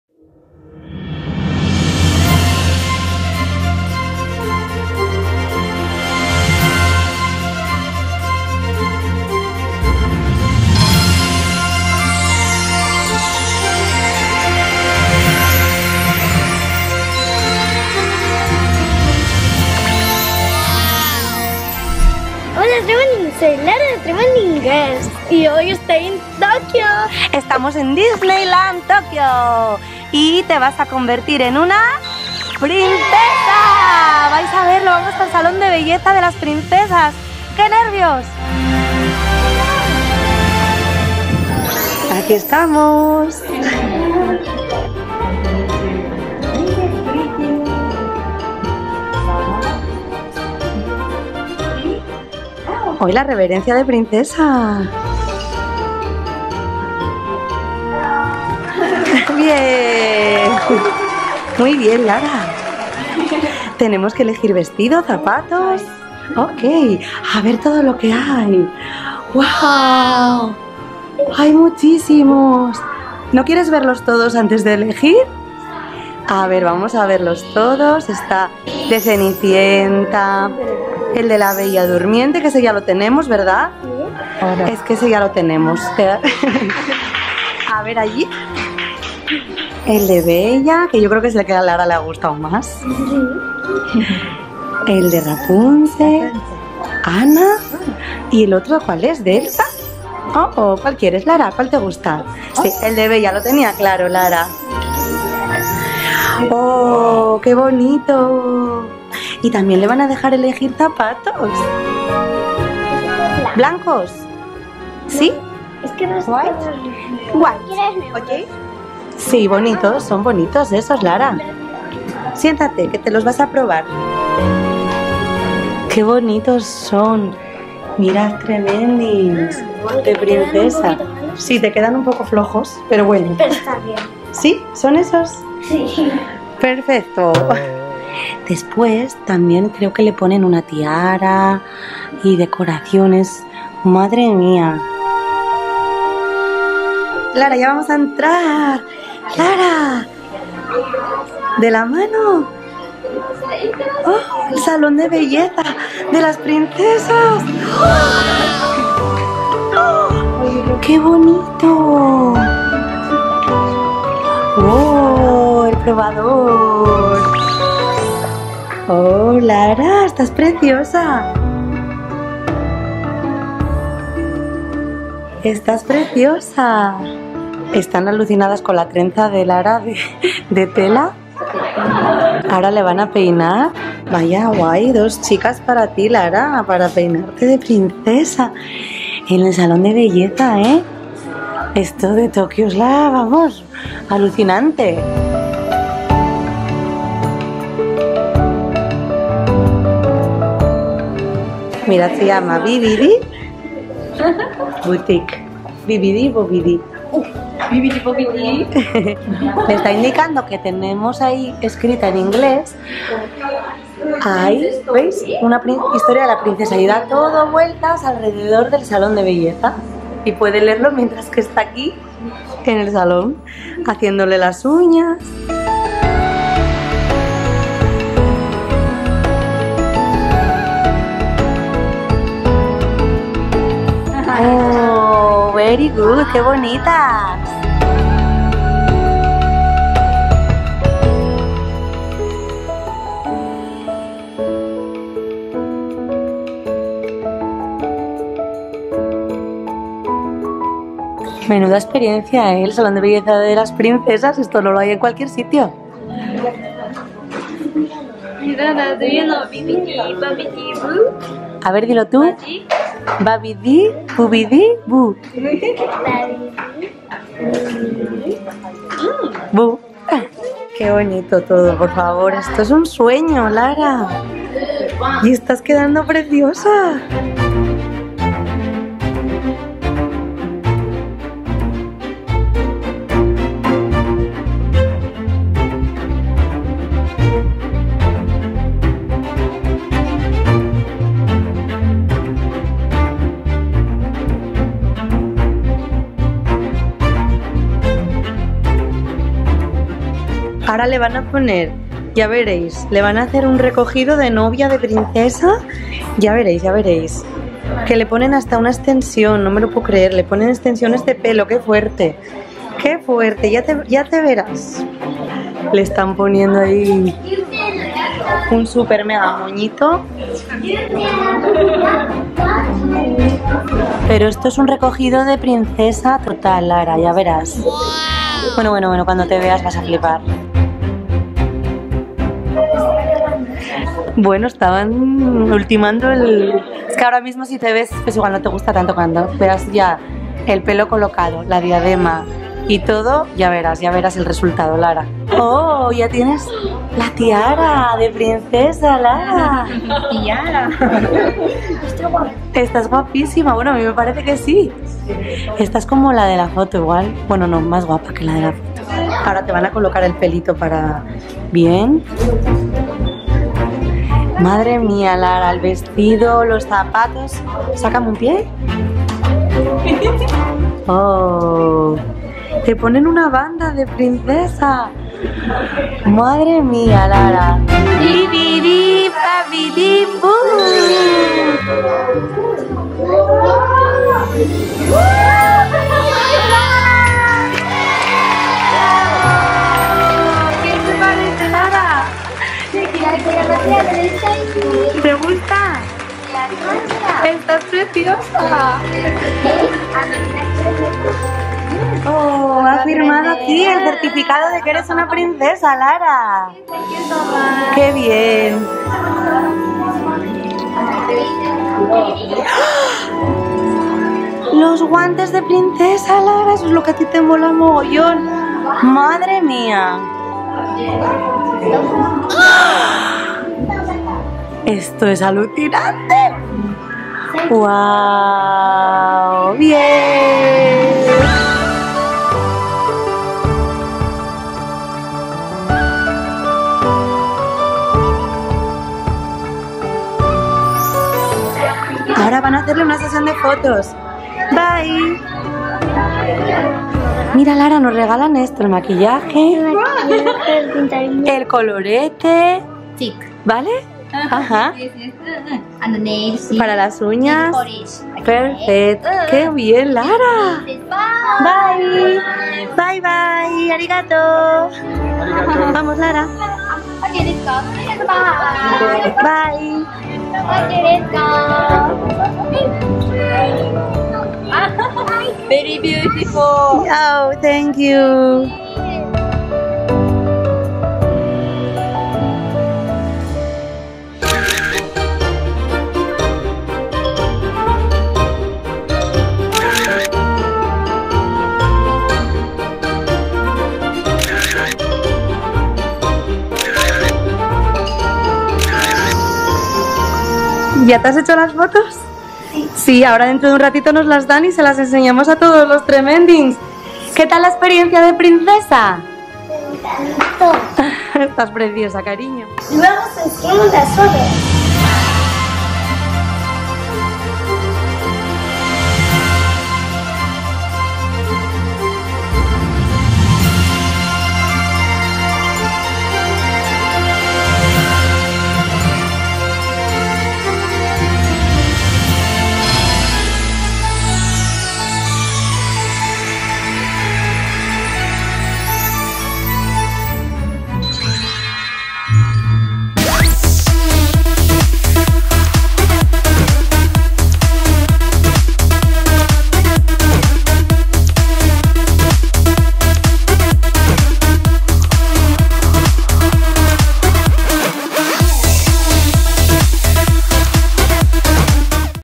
Me convierto en PRINCESA DISNEY en Disneyland Tokio - Viaje Tremending
Me visto de BELLA y me maquillan y peinan como una AUTÉNTICA PRINCESA DISNEY en Disneyland Tokio.

❤ ¡Os quiero porque sois Tremendings! ❤

💌 DIRECCIÓN POSTAL:
Apartado de Correos 9005
33212 Asturias
España

Este canal esta grabado, editado y supervisado por adultos.